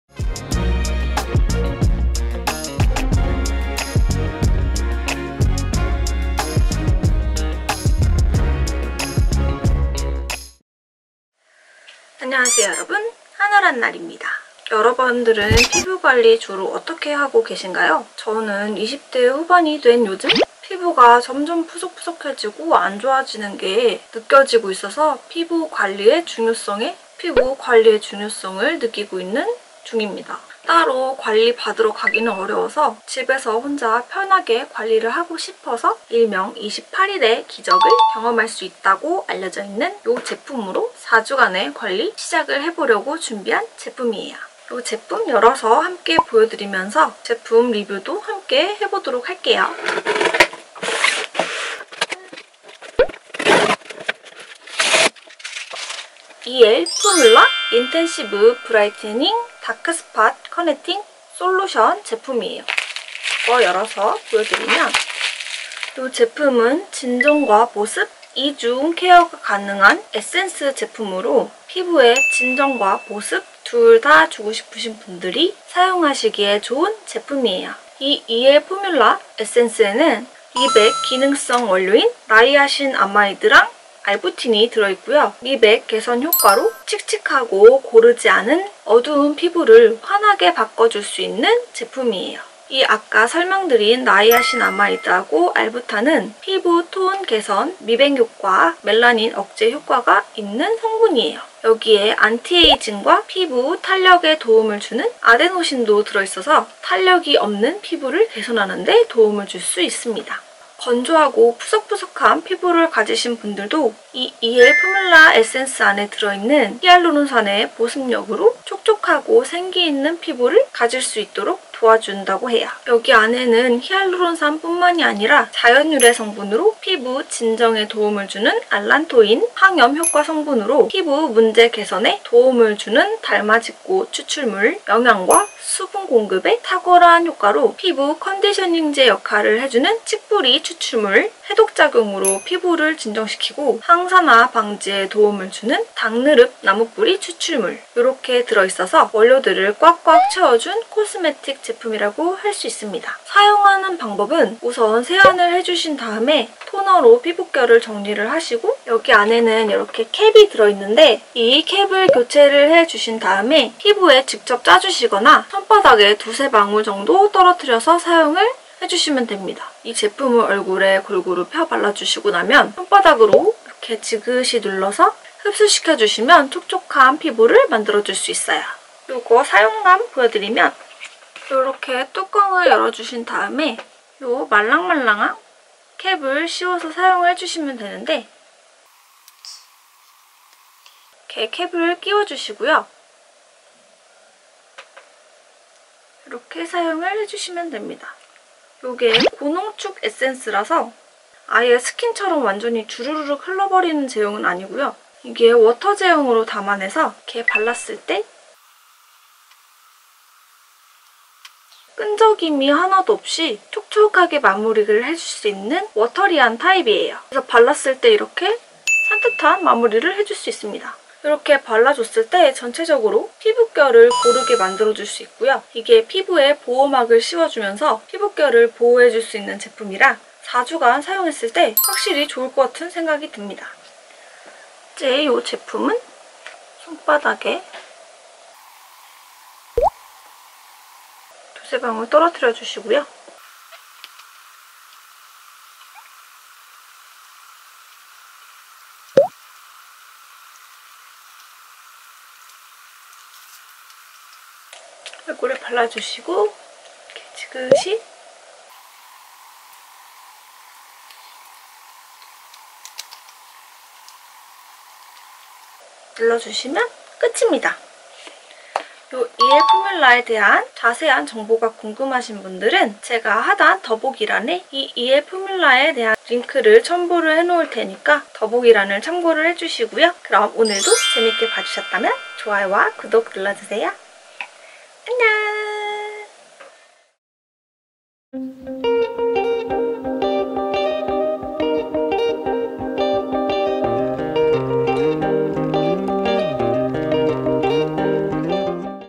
28일의기적 이엘포뮬라로 밝은피부와 수분보충 모두 케어하기!
안녕하세요! 한얼한날입니다 :)
저는 피부가 푸석푸석한 편이기도 하고
피부에 수분감이 없는 편이라서
두가지 모두 피부고민이 있는 편인데
밝은피부와 수분보충 모두를 케어해줄 수 있는
일명 28일의기적이라고 불리는 제품이 있어서
오늘의 추천템으로 가져왔어요!

이엘포뮬라 직접 사용 해 본 리얼후기입니다
오늘 영상도 재미있게 봐주세요